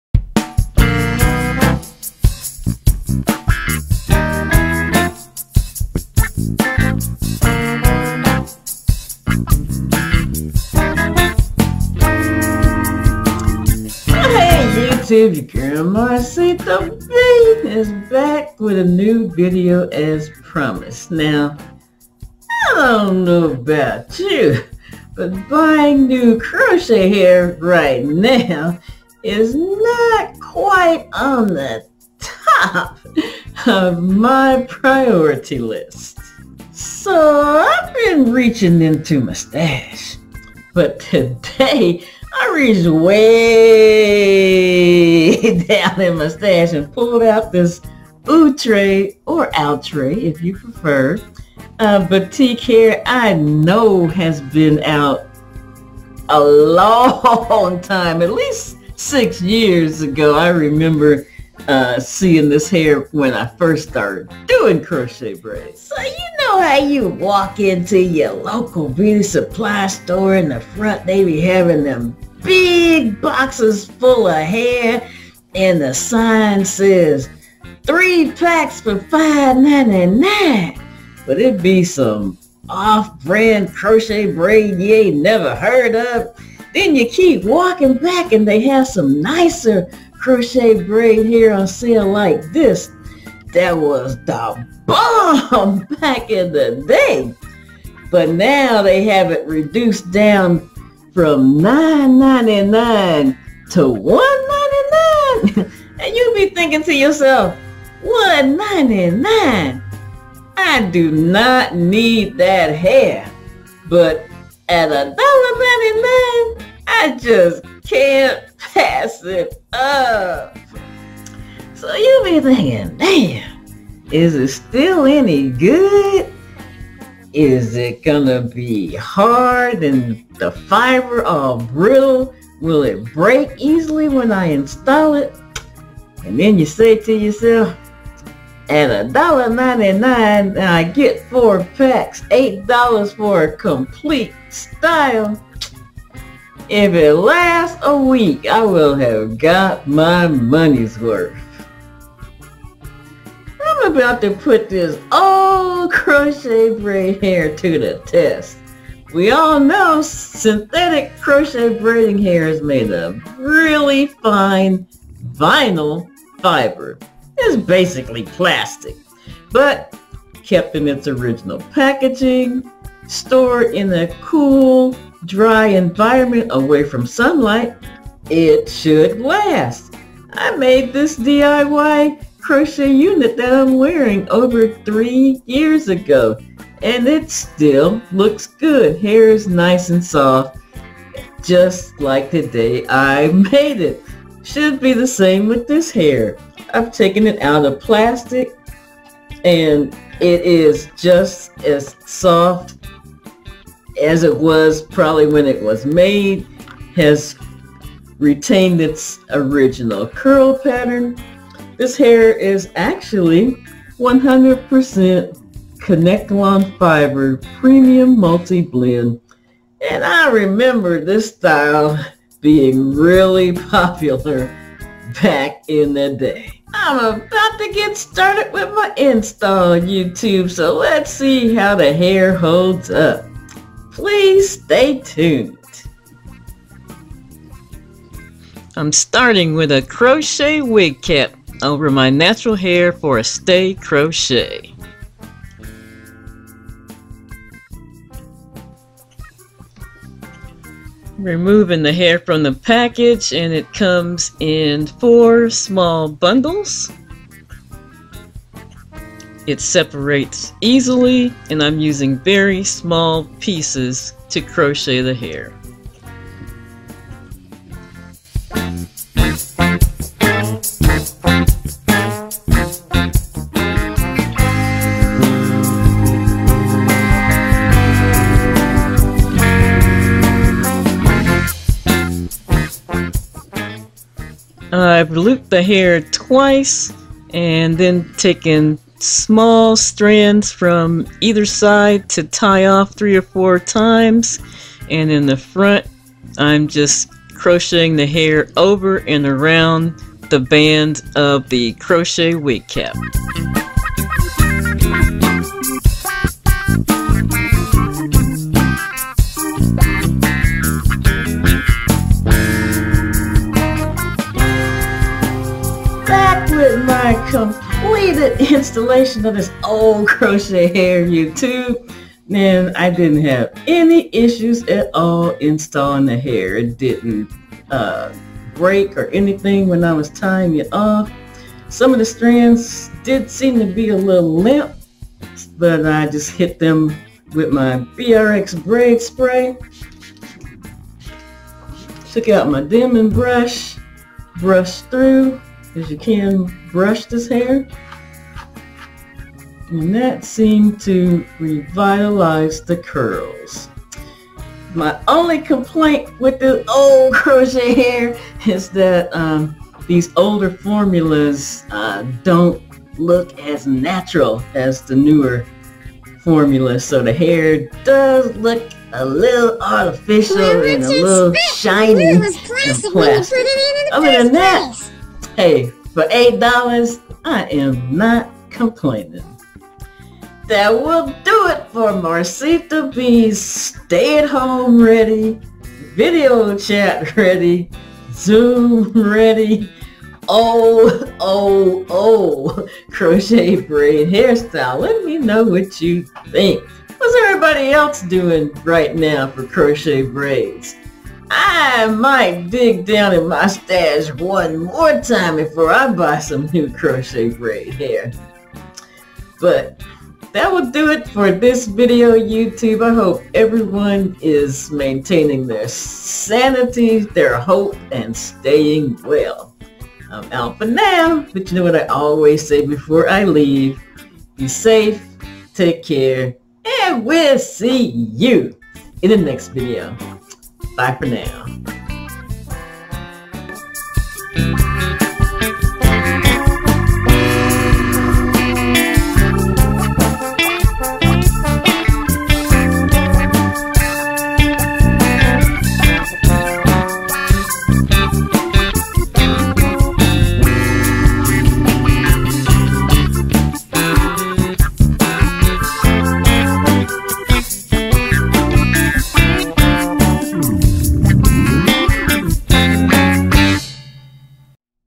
ZOOM READY CROCHET BRAIDS $1.99 A PACK. DOES OLD CROCHET HAIR EVER EXPIRE?
Does Crochet Hair have a shelf life?
Even this old, old Outre Batik Romance Curl is still usable and looks good
6 years later. A few online site are actually still selling this style: